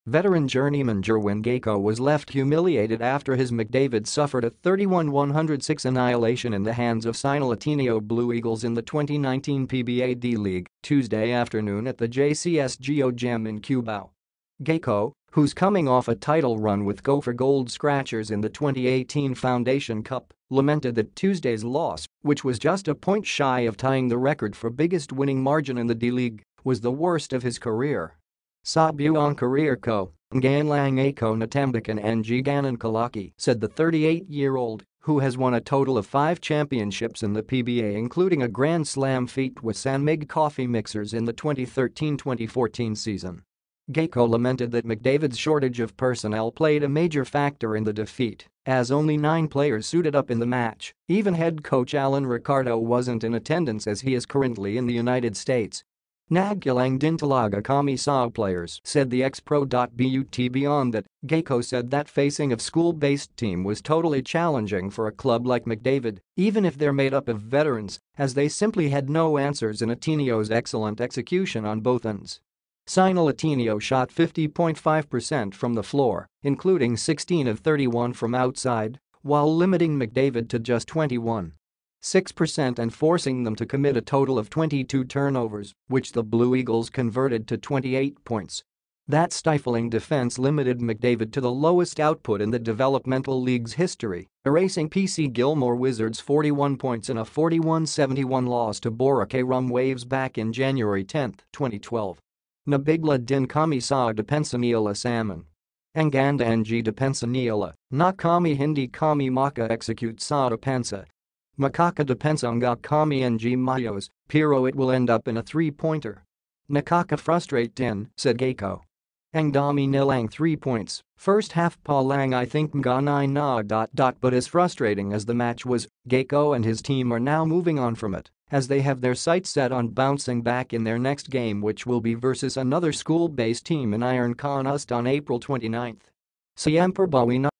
Jerwin Gaco left disgusted after McDavid’s 75-point loss: ‘Ngayon lang ako natambakan ng ganun’
Veteran journeyman Jerwin Gaco was left humiliated after his McDavid suffered a 31-106 annihilation in the hands of Cignal-Ateneo Blue Eagles in the 2019 PBA D-League, Tuesday afternoon at the JCSGO Gym in Cubao.

Gaco, who’s coming off a title run with Go for Gold Scratchers in the 2018 Foundation Cup, lamented that Tuesday’s loss — which was just a point shy of tying the record for biggest winning margin in the D-League — was the worst of his career.

“Sa buong career ko, ngayon lang ako natambakan ng ganun kalaki,” said the 38-year-old, who has won a total of five championships in the PBA including a Grand Slam feat with San Mig Coffee Mixers in the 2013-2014 season.

Gaco lamented that McDavid’s shortage of personnel played a major factor in the defeat, as only nine players suited up in the match — even head coach Allen Ricardo wasn’t in attendance as he is currently in the United States.

“Nagkulang din talaga kami sa players,” said the ex-pro.

But beyond that, Gaco said that facing a school-based team was totally challenging for a club like McDavid even if they’re made up of veterans, as they simply had no answers in Ateneo’s excellent execution on both ends.

Cignal-Ateneo shot 50.5 percent from the floor — including 16-of-31 from outside — while limiting McDavid to just 21.6 percent and forcing them to commit a total of 22 turnovers, which the Blue Eagles converted to 28 points.

That stifling defense limited McDavid to the lowest output in the developmental league’s history, erasing PC Gilmore Wizards’ 41 points in a 41-71 loss to Boracay Rum Waves back in January 10, 2012.

“Nabigla din kami sa depensa nila samin. Ang ganda ng depensa nila, na kami hindi kami maka-execute sa opensa. Makaka-depensa nga kami ng maayos, pero it will end up in a three-pointer. Nakaka-frustrate din,” said Gaco.

“Ang dami nilang three-points, first half pa lang I think mga [nine] na.”

But as frustrating as the match was, Gaco and his team are now moving on from it, as they have their sights set on bouncing back in their next game which will be versus another school-based team in Ironcon-UST on April 29.

“Siyempre bawi na lang next game.”